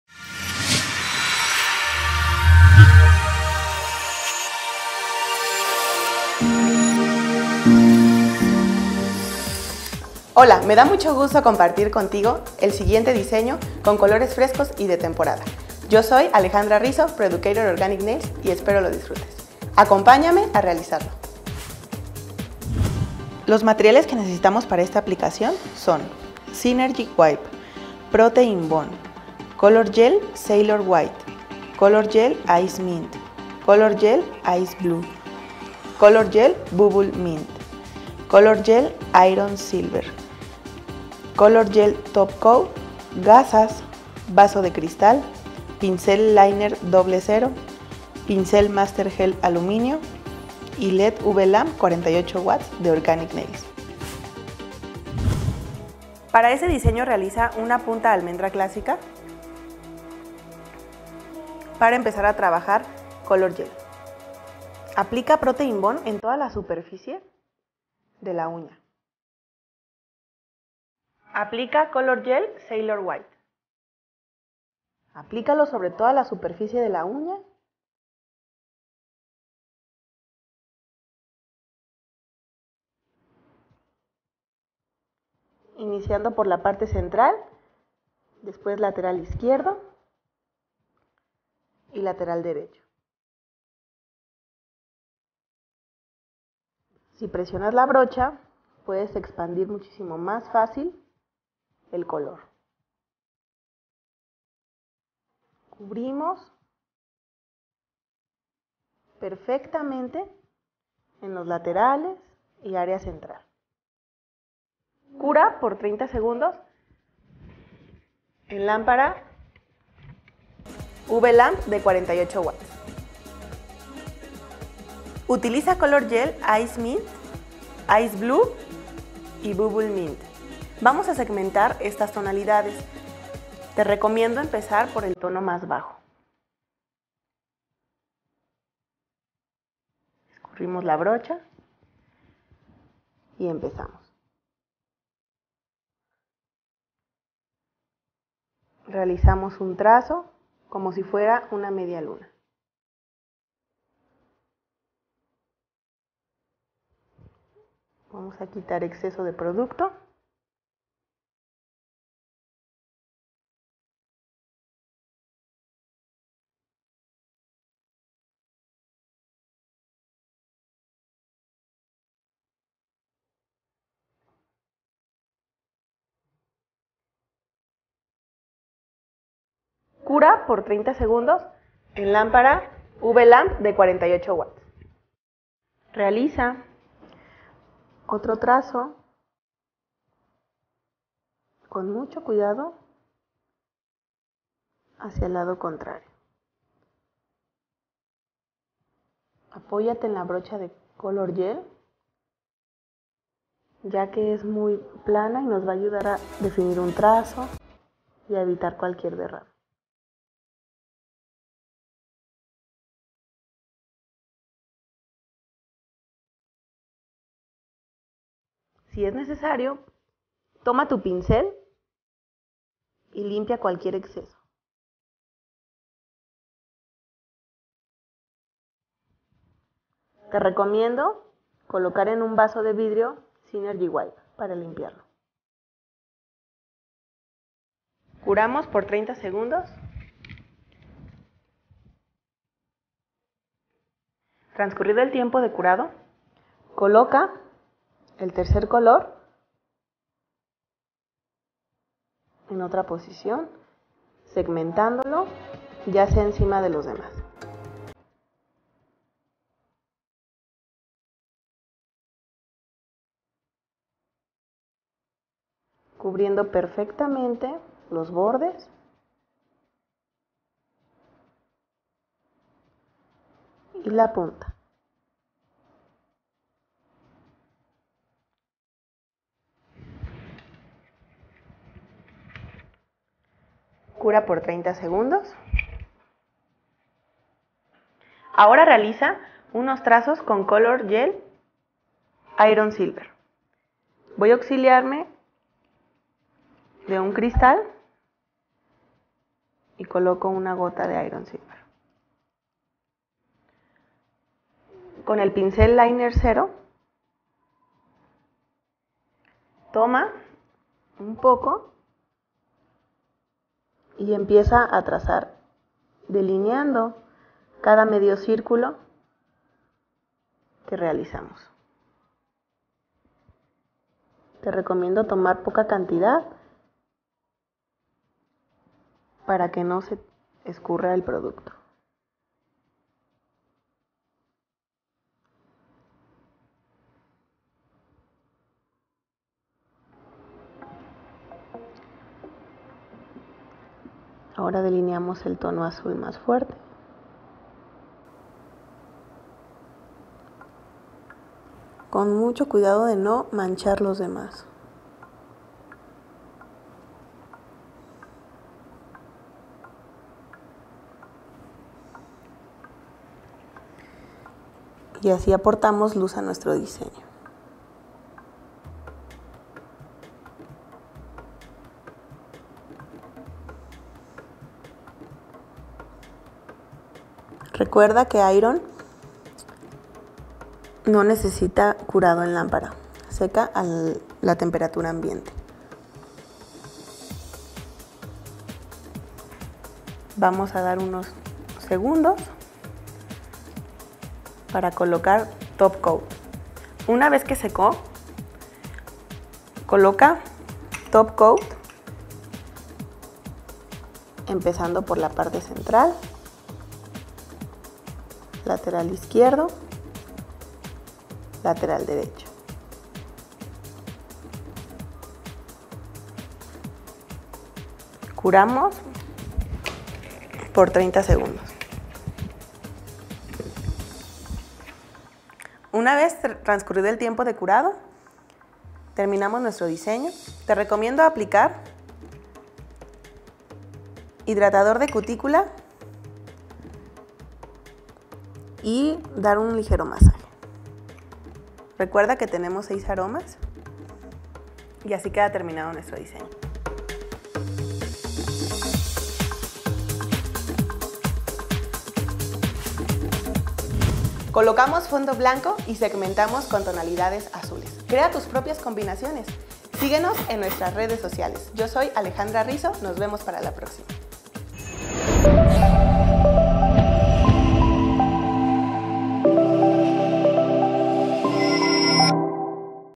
Be Patient / Color Gel
Alejandra Riso / Pro Educator Organic Nails

Preparación
• Lima 150/150
• Synergy Wipe
• Protein Bond
• Gasas
• Natural Cover Peach

Aplicación
•Vaso De Crystal
•Color Gel Sailor White
•Color Gel Ice Mint
•Color Gel Ice Blue
•Color Gel Bubble Mint
•Color Gel Iron Silver
•Pincel Liner 00
•Pincel Master Gel Aluminio

Terminado
• Top Coat Gel
• Led/uv Lamp 48 W Organic Nails
• Aceite para cutícula